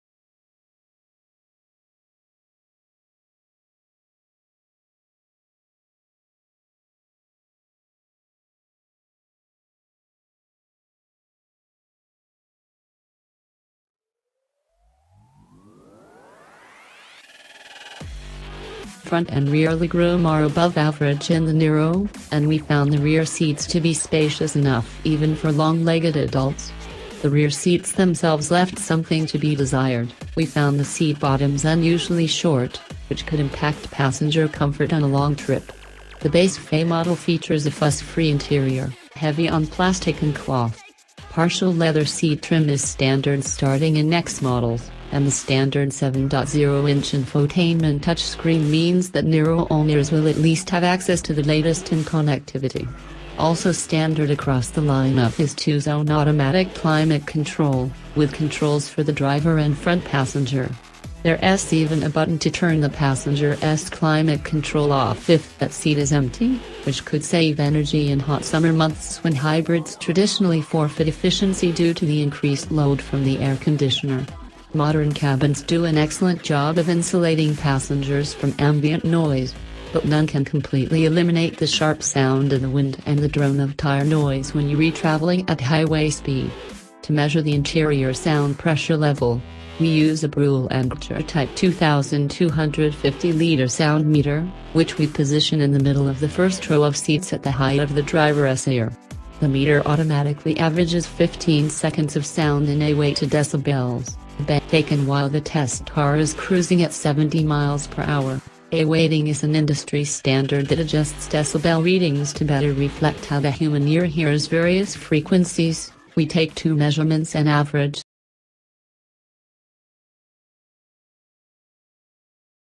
Hot News !!! 2018 Kia Niro Interior and Infotainment Overview
██████ GET DAILY UPDATES! NOW! ██████